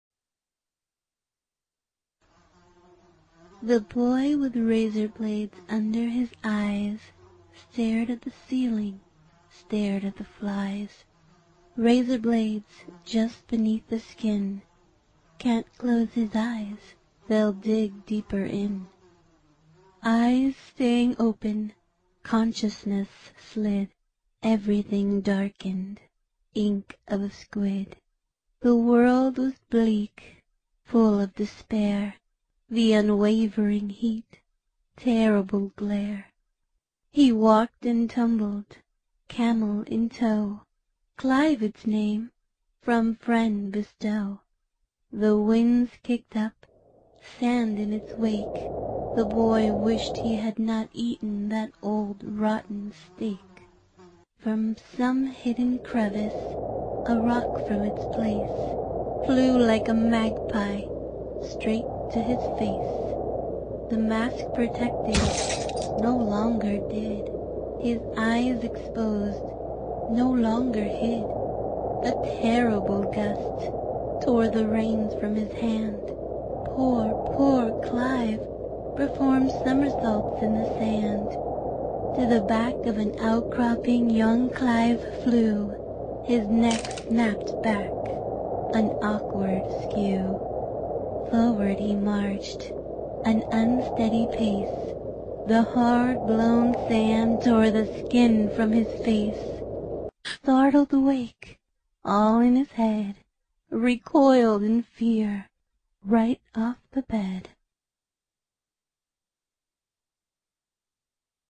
the boy...."Sandstorm" remake
this is a remake of the first episode in the series. this episode was remade to further expand on the story and because I didn't really like the original.